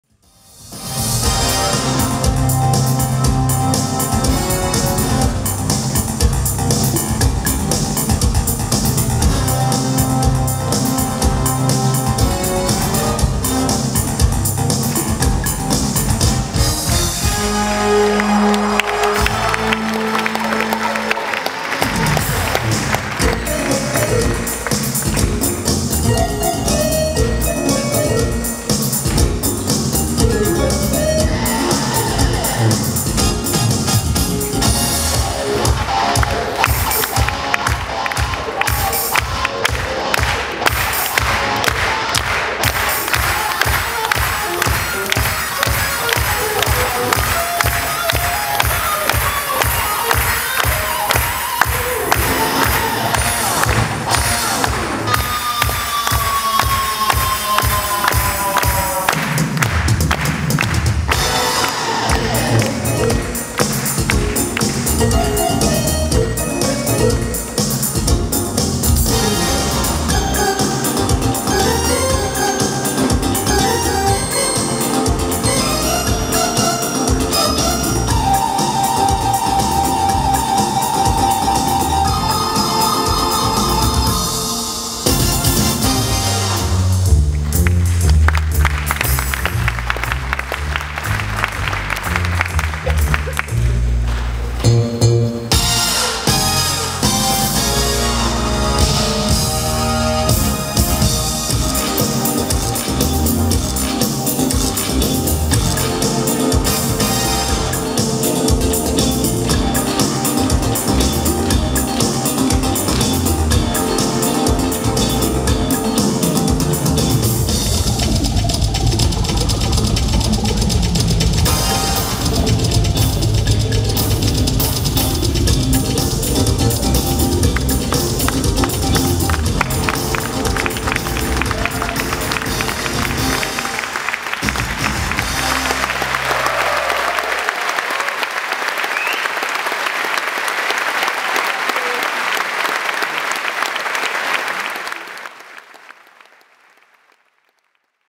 Besen-Nummer
Besen-Nummer von Romano Carrara am Internationalen Varietéfestival in Sennfeld (Deutschland)
Video: Werner Buhl